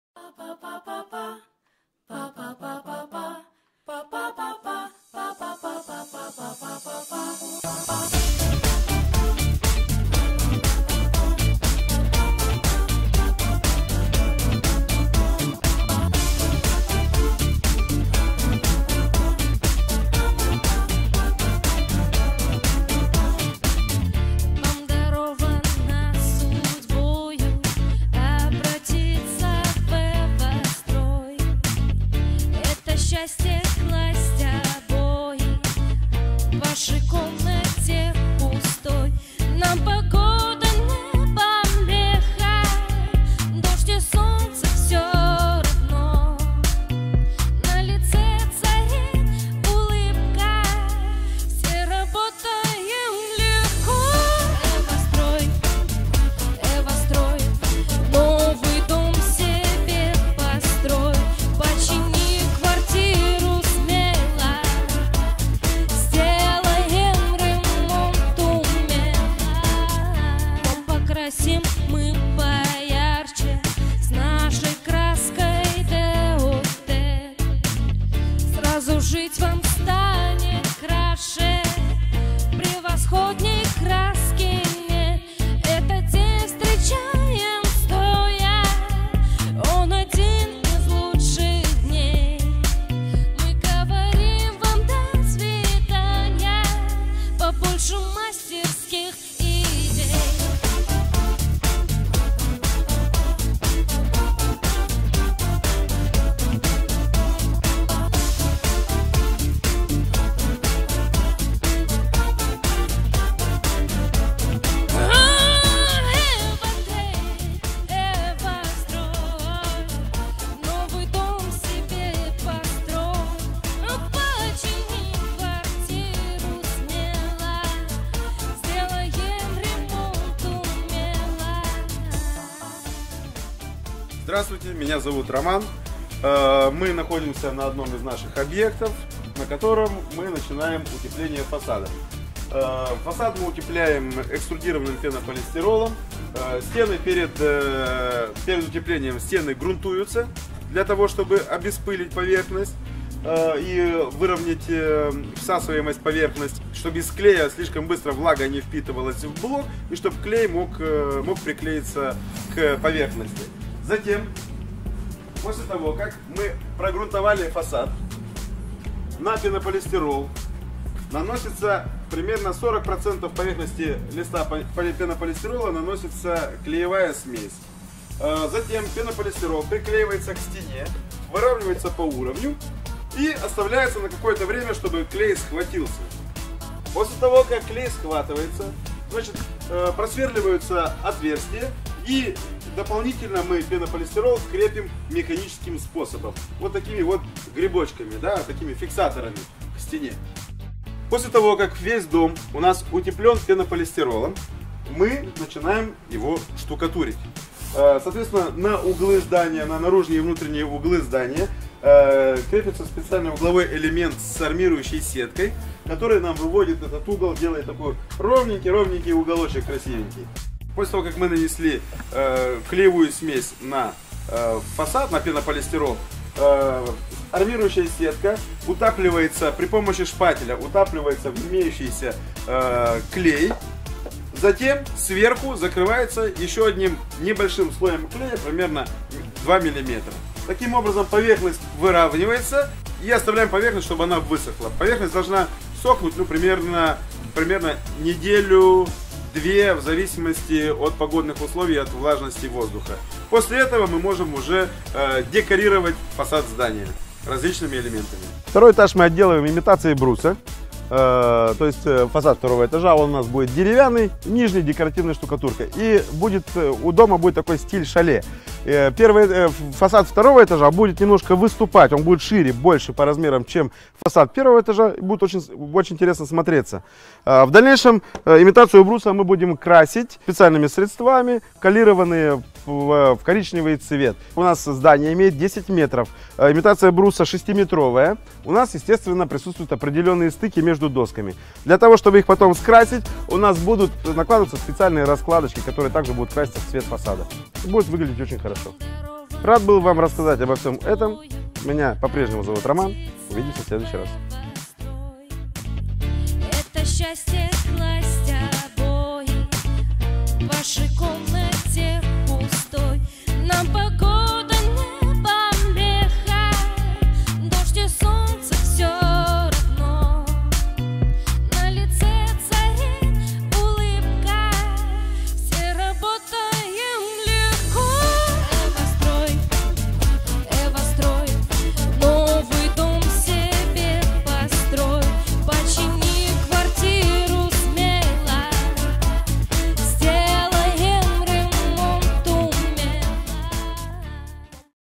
СТРОИТЕЛЬСТВО БАНИ + ИНСТРУКЦИЯ ЭВАСТРОЙ
В нашем ролике, мы продемонстрируем Вам поэтапное строительство цокольного этажа.

ОТЗЫВ ЗАКАЗЧИКА О ЭВАСТРОЙ - СТРОИТЕЛЬСТВО ДОМА ( Перекрытие. Часть - II )
- ВОТ КАК НАДО СТРОИТЬ ДОМ !!!